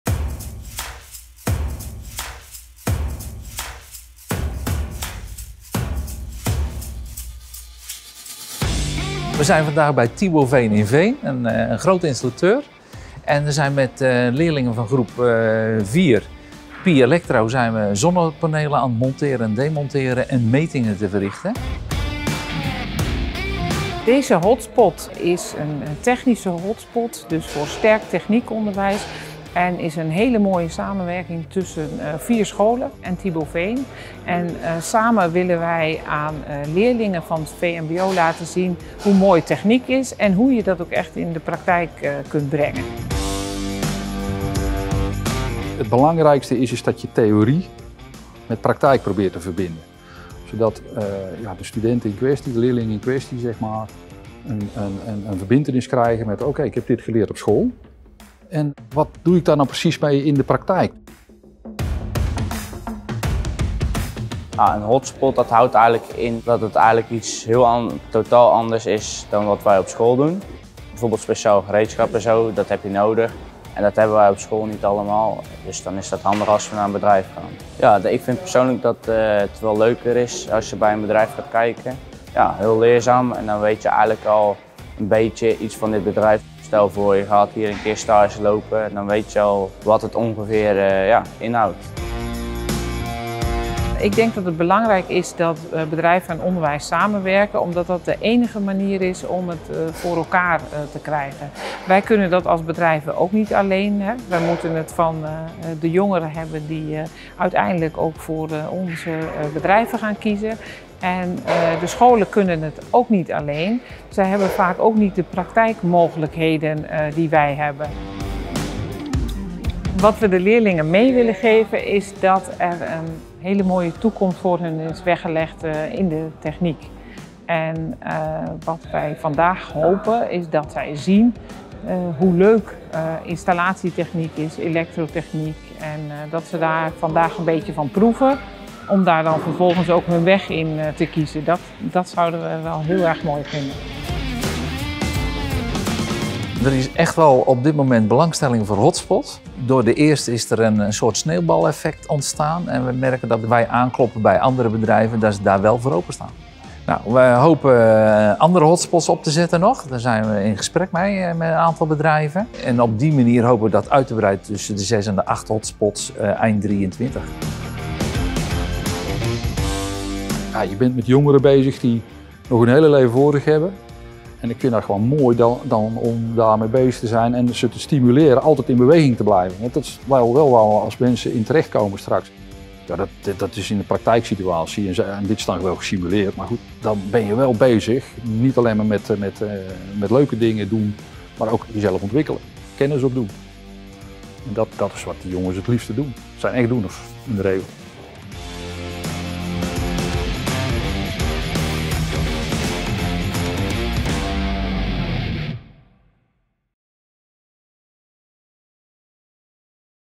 Techniek Hotspot Tibo-Veen
Vierdejaars leerlingen Elektro van het Gilde Vakcollege Techniek gaan aan het werk bij Installateur Tibo-Veen in Veen waar ze zonnepanelen gaan monteren en demonteren en metingen verrichten. Deze hotspot is een technische hotspot voor Sterk Techniekonderwijs en is een hele mooie samenwerking tussen 4 scholen en Tibo-Veen. Theorie wordt verbonden met de praktijk. Gezamenlijk hebben zij de ambitie om het leren op de bedrijfswerkplek meer te integreren in het technisch vmbo. Theorie wordt verbonden met de praktijk. Samen willen zij aan leerlingen in het vmbo laten zien hoe mooi techniek is en hoe je dat ook echt in de praktijk kunt brengen.